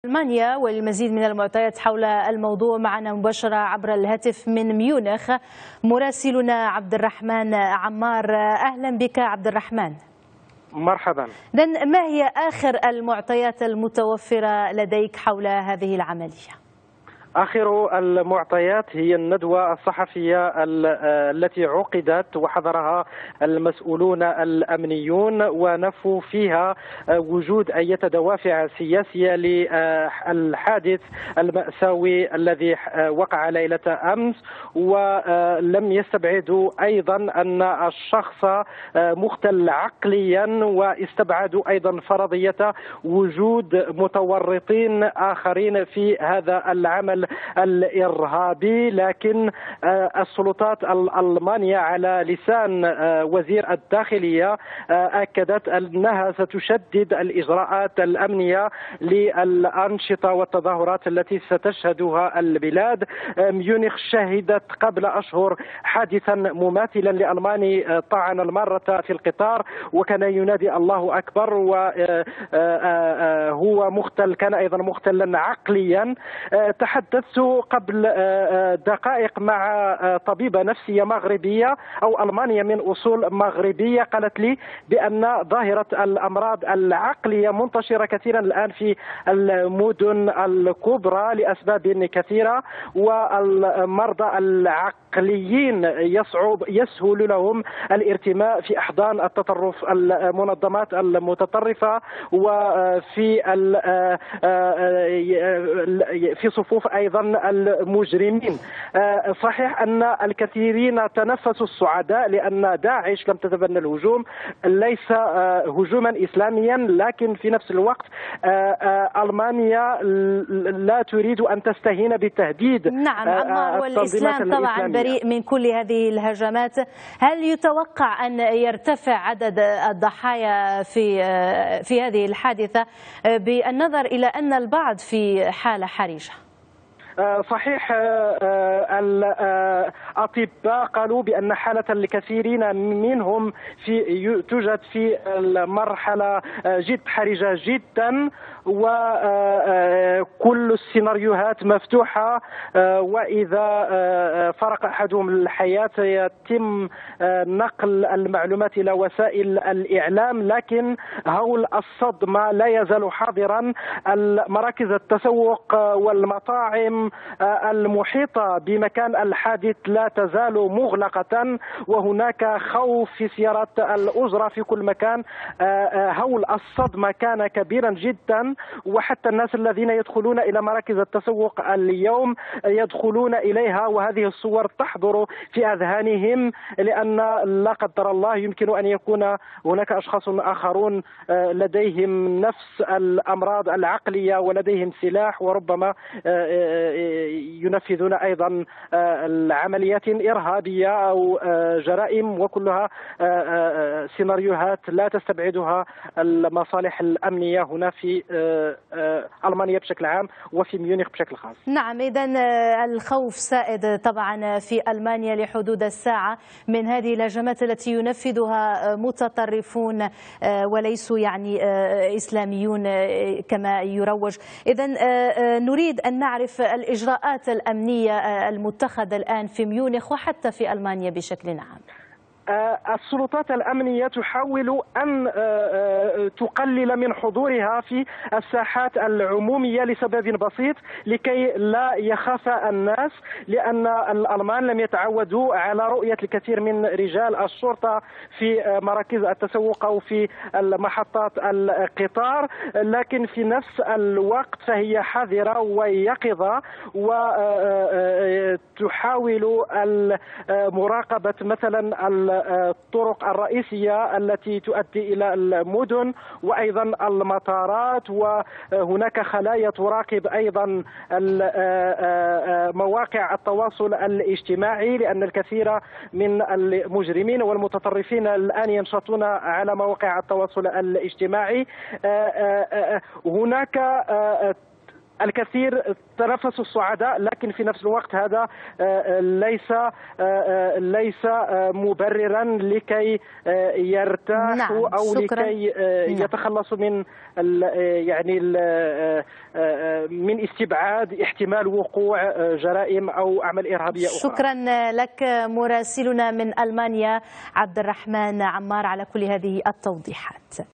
ما هي الإجراءات التي اتخذتها السلطات الألمانية بعد حادث ميونيخ؟
يتحدث عبد الرحمان عمار، مراسل مدي 1 تيفي من ألمانيا، عن آخر التطورات بعد الهجوم الذي هز ولاية "بافاريا" الجمعة 22 يوليوز، والذي خلف 10 قتلى وعدد من الإصابات.